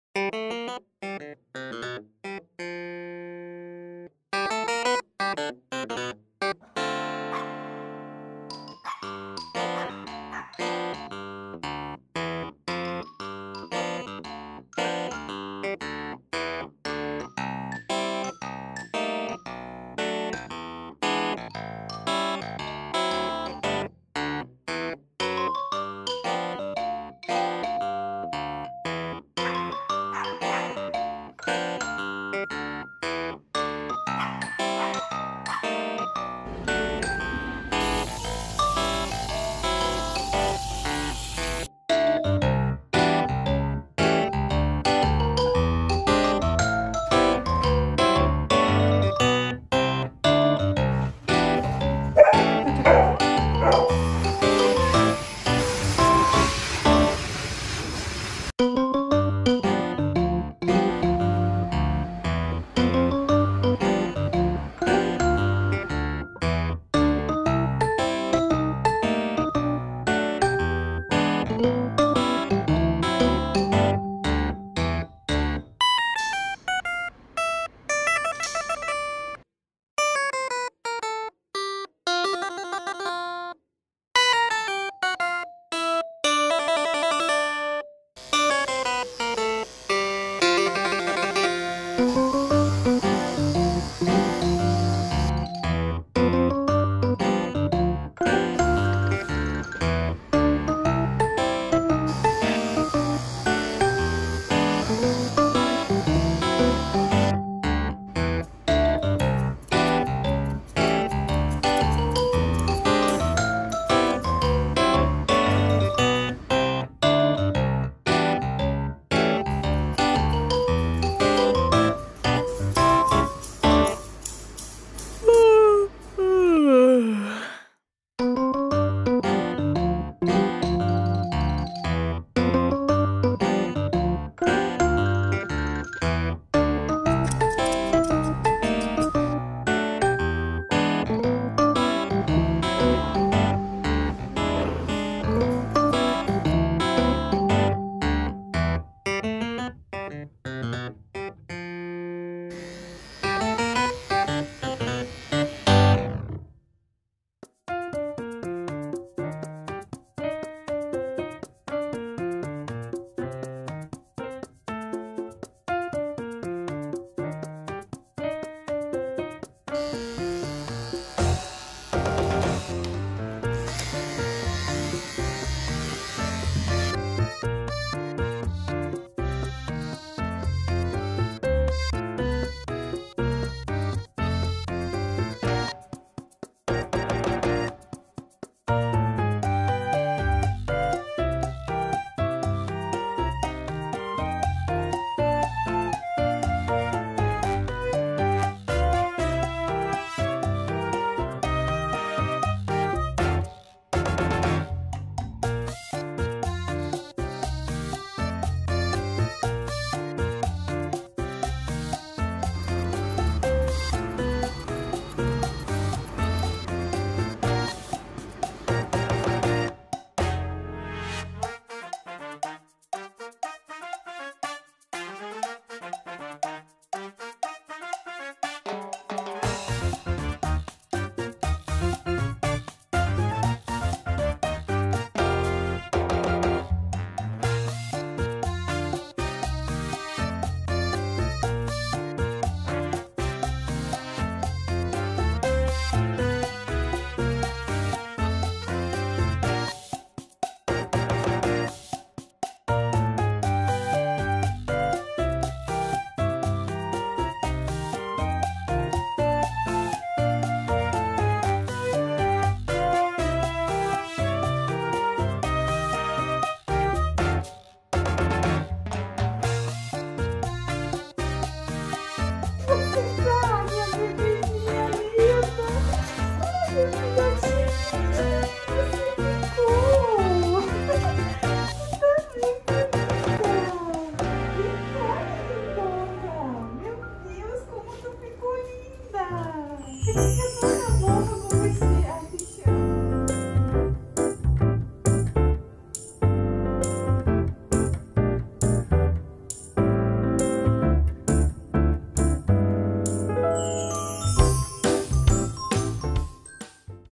SHIHTZU PUPPY, FIRST GROOMING WITH SCISSOR ✂️❤️🐶 cuteness guaranteed!
Korean Grooming Techniques!
Total transformation of a shihtzu with grooming. shihtzu baby cut, cuteness guaranteed!

Hi, I'm Layra, a 6 month old shihtzu puppy!! Today was my first grooming with scissor at Tosa Bebe Pet Center. What did you think? Did I get cuter still?? kisses✂️✂️✂️💋💋🐶